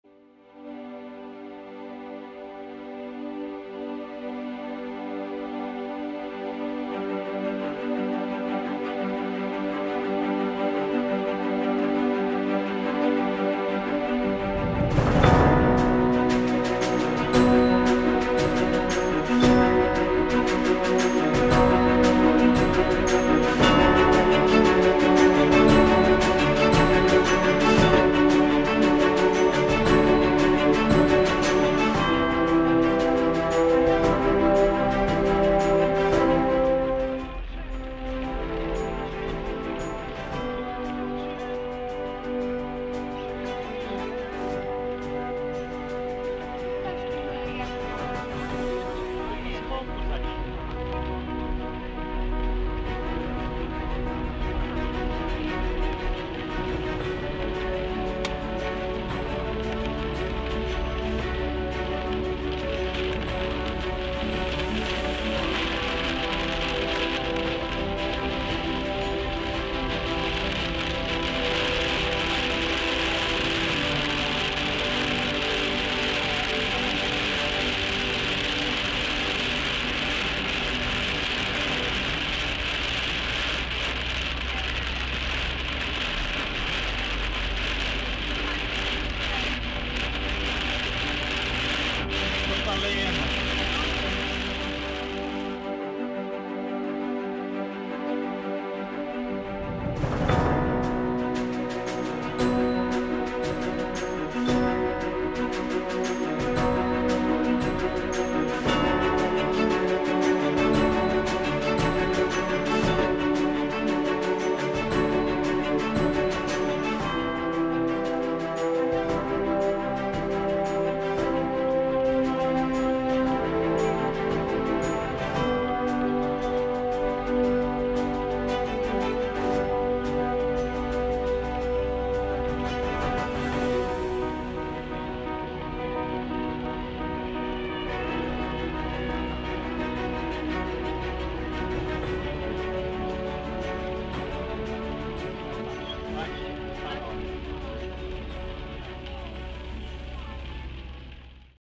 Nagła zmiana warunków na drodze i końcowy efekt.
Nie każdy dostosował się do nagłej zmiany warunków na drodze.